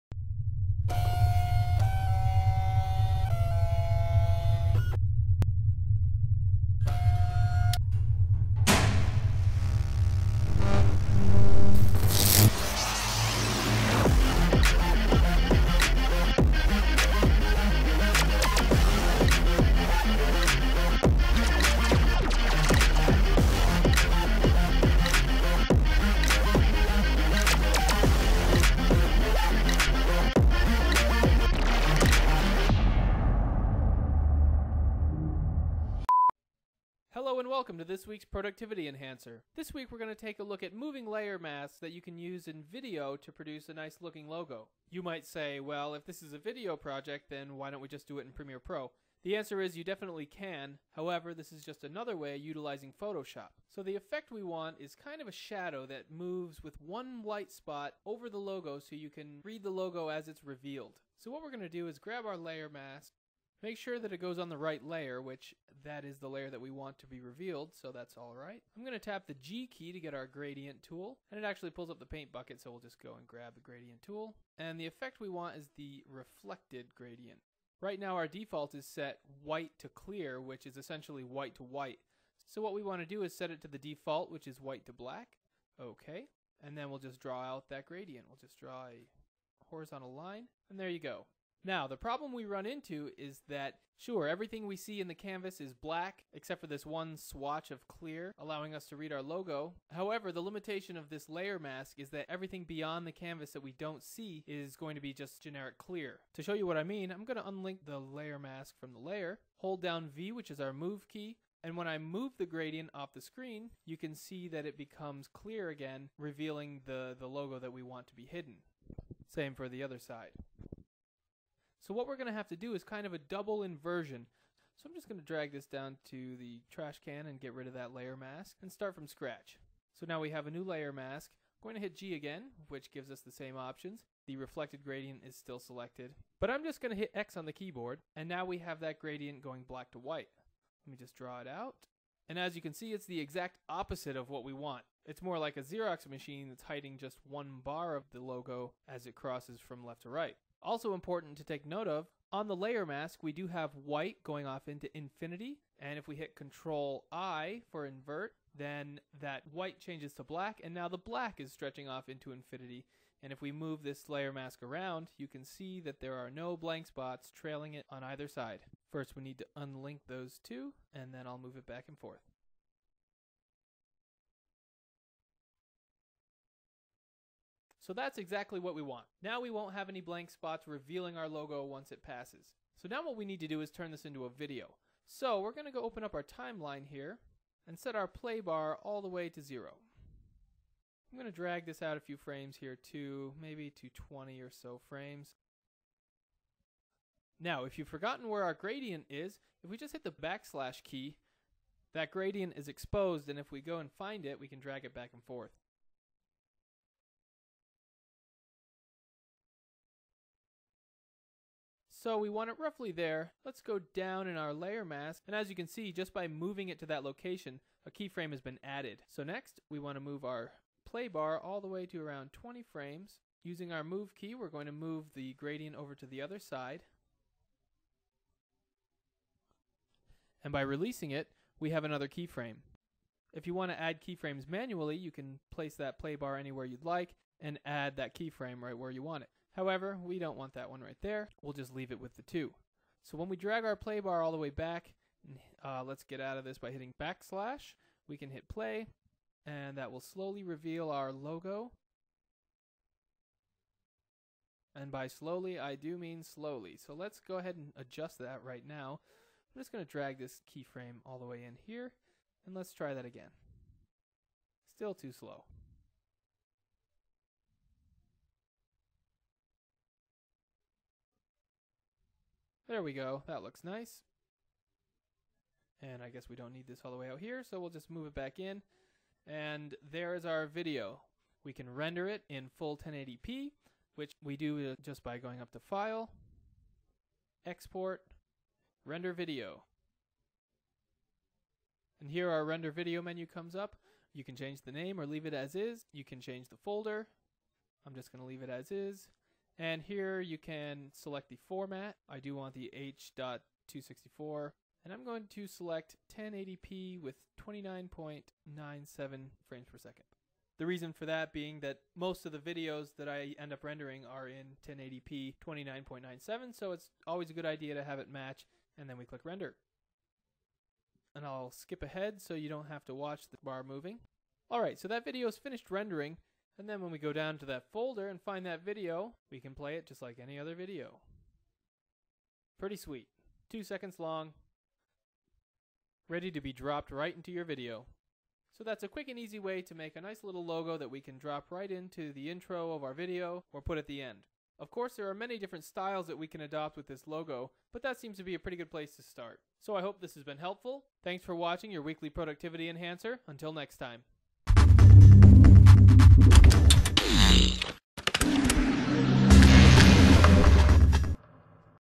Weekly Productivity Enhancer: Moving Layer Masks in Photoshop
Creating an eye catching logo to tag onto the end of an introduction, or even to end your video is always a classy move. You might want to do this in Premiere Pro, but Adobe likes to give you as many options as possible within all programs of the Creative Suite. This video walks you through the steps of making that logo in Photoshop by using a moving layer mask.